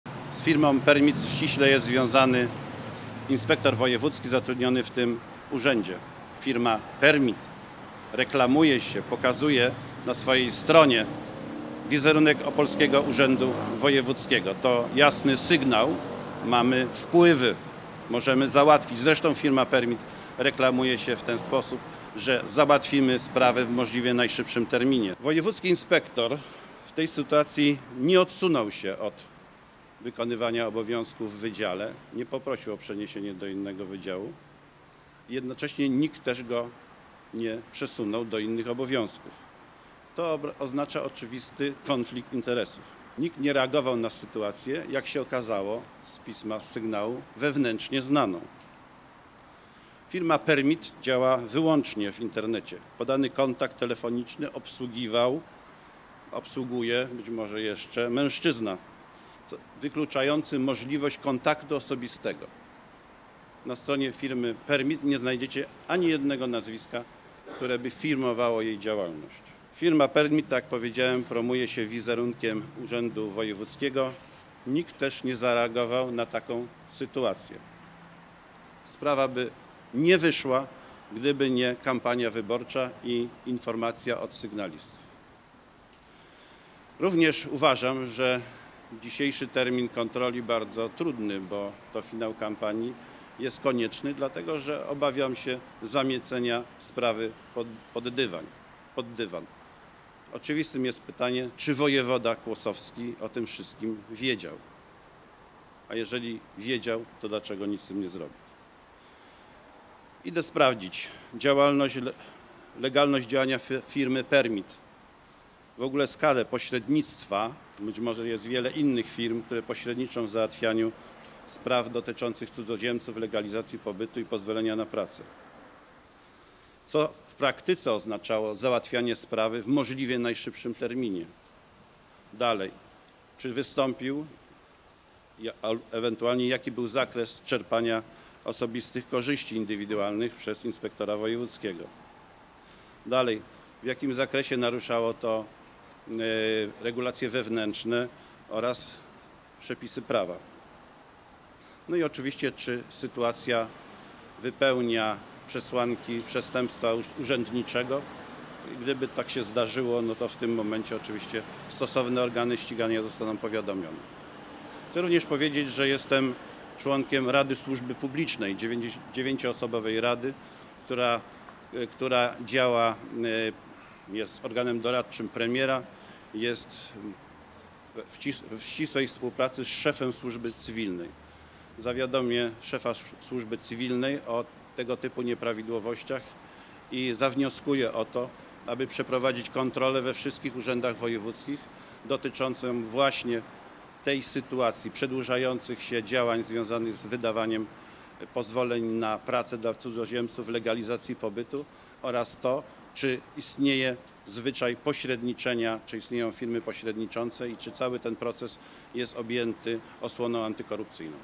"Afera wizowa" w Urzędzie Wojewódzkim
Zdaniem kandydata Koalicji Obywatelskiej w wyborach do Sejmu, w Urzędzie mogło dochodzić do sytuacji, w której jeden z inspektorów mógł „działać w warunkach konfliktu interesów”. Czego miał dokładanie dotyczyć problem? O szczegółach mówił na dzisiejszej (13.10) konferencji prasowej Ryszard Wilczyński z PO.